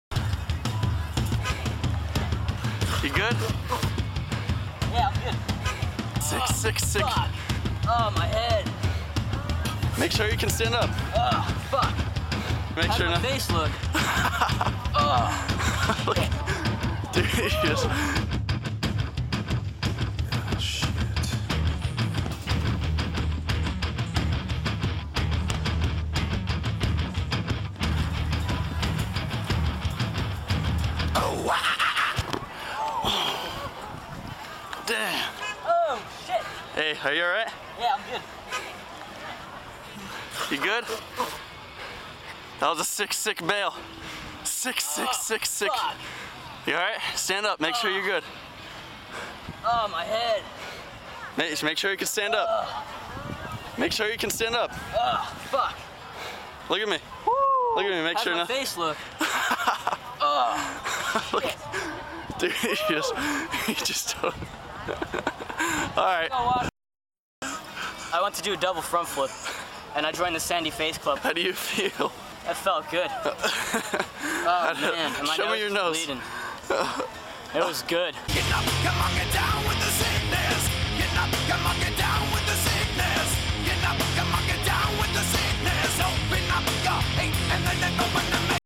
Julian Vazquez double front to face!
Well, I guess I have to try again in a few days when my neck feels better.